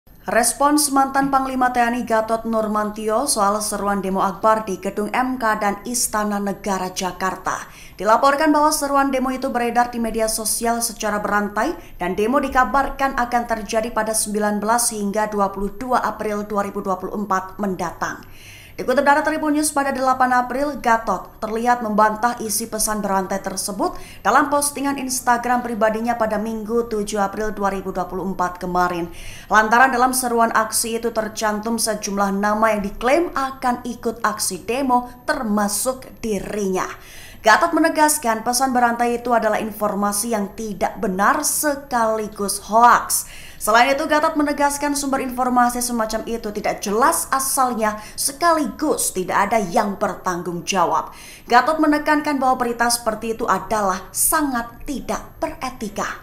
Seruan Demo Akbar di Gedung MK dan Istana Negara Jakarta Direspons Eks Panglima TNI Gatot Nurmantyo
TRIBUN-VIDEO.COM - Respons mantan Panglima TNI Gatot Nurmantyo soal seruan demo akbar di gedung Mahkamah Konstitusi (MK) dan Istana Negara Jakarta.